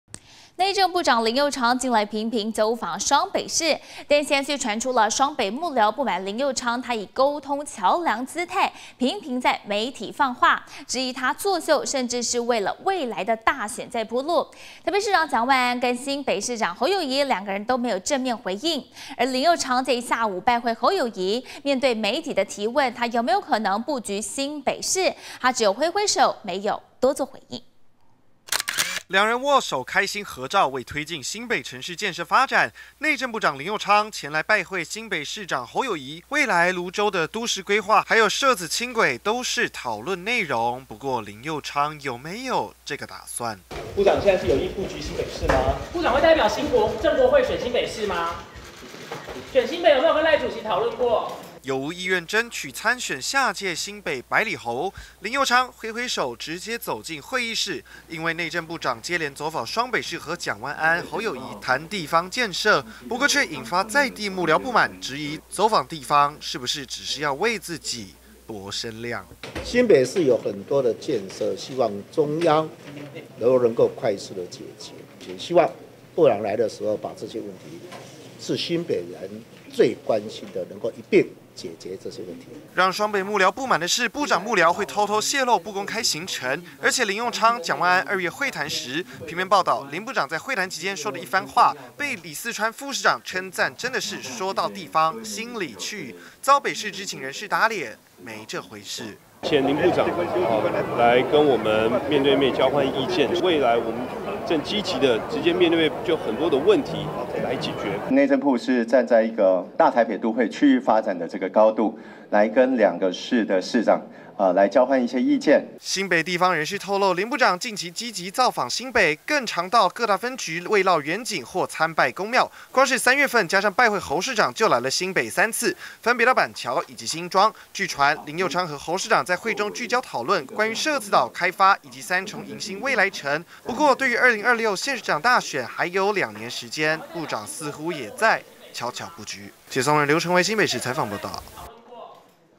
勤走雙北布局抬聲量？ 林右昌揮手微笑不回應
內政部長林右昌近來頻頻走訪雙北市，但現在卻傳出，雙北幕僚不滿林右昌以「溝通橋梁姿態」頻頻在媒體放話，質疑他做秀甚至是為未來大選鋪路！台北市長蔣萬安，以及新北市長侯友宜，兩人都沒有正面回應，僅喊話中央，不論是社子島、又或者是105市道改善工程，中央可以一起面對，而林右昌下午拜會侯友宜，面對媒體提問有無布局新北市可能？他僅揮揮手微笑，沒有多做回應。